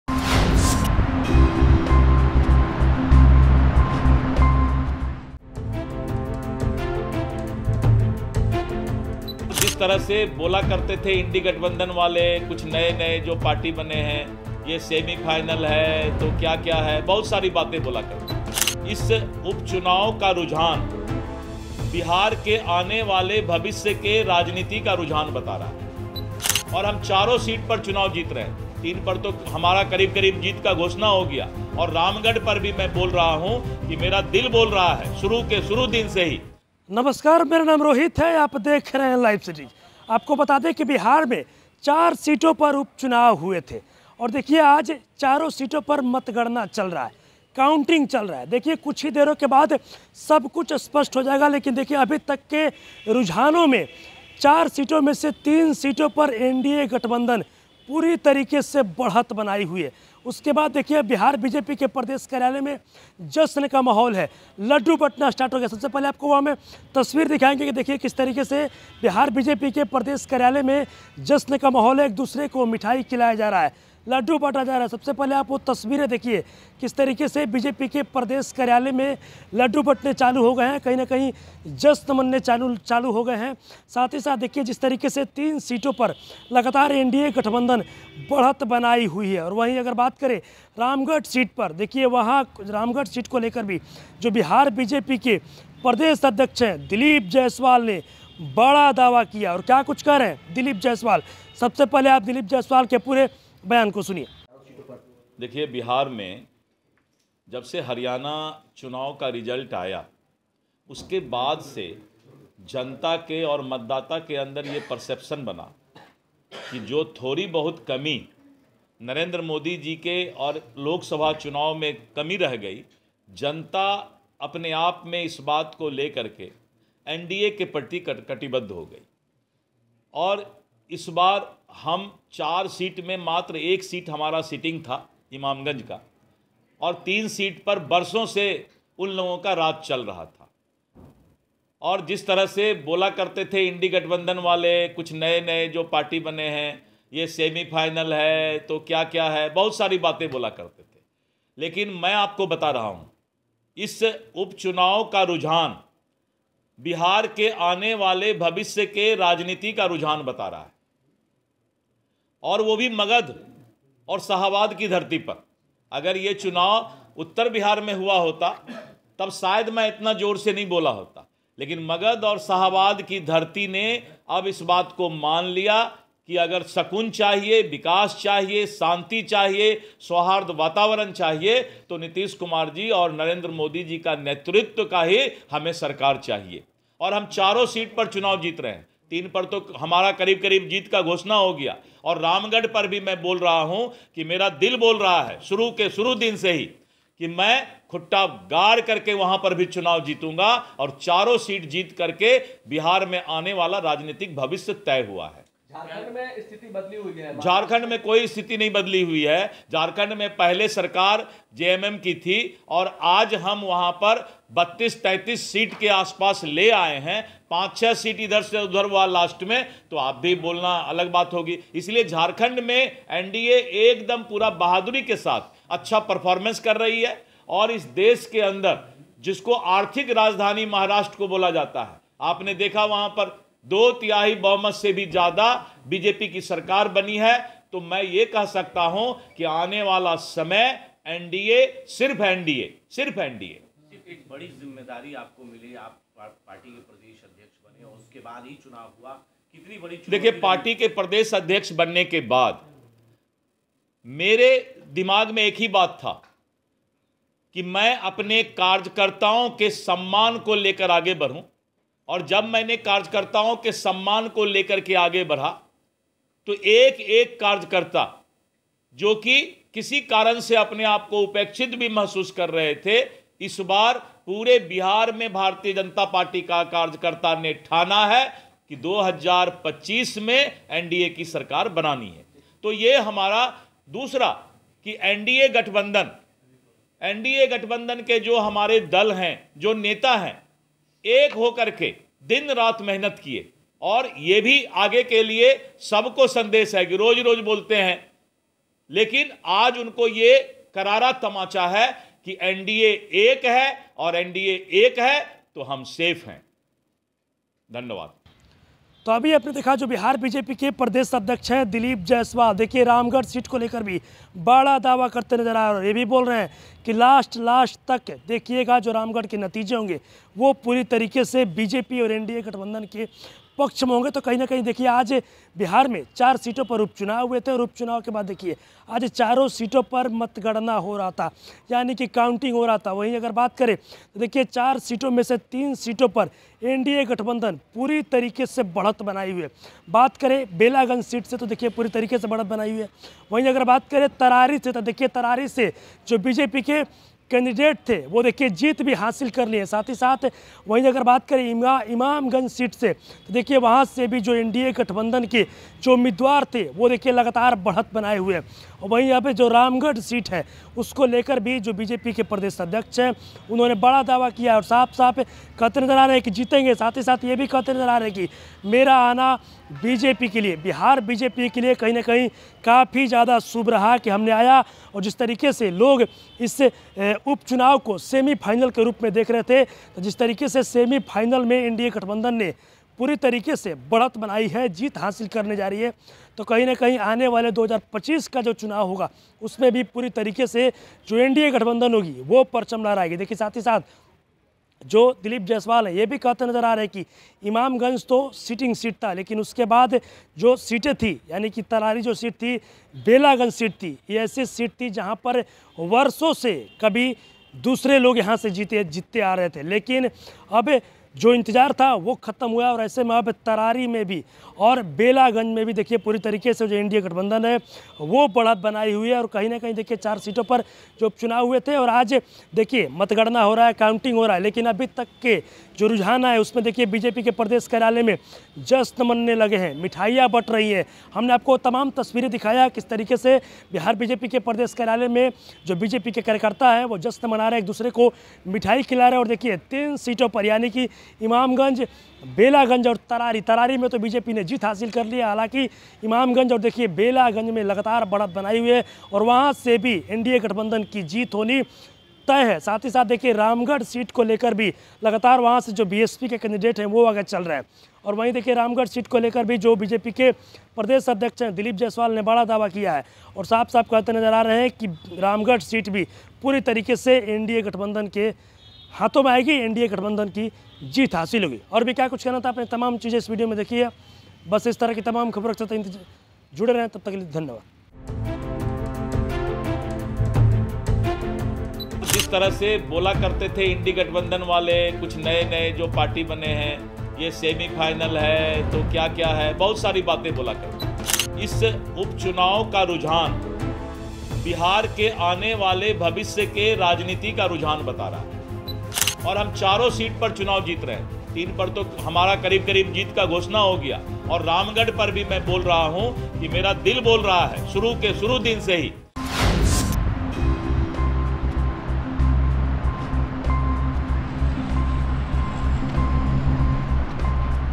Bihar Bypoll Results : Result आते ही BJP Office में जश्न का माहौल, खूब बंट रहे लड्डू, RJD हवा में..
Bihar Bypoll Results : Result आते ही BJP Office में जश्न का माहौल, खूब बंट रहे लड्डू, RJD हवा में | Bihar News | Live Cities